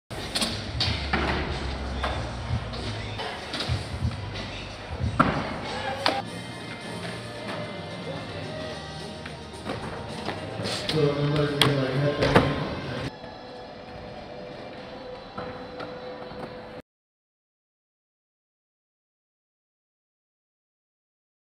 Hayley Wilson at BAYSIXTY6 | SLS World Tour | World Skate - Global Open Qualifier (2019)
Hayley Wilson at BAYSIXTY6 Skate Park during the SLS World Tour | World Skate - Global Open Qualifier (2019).

Filmed & edited by Luke Parker for BAYSIXTY6.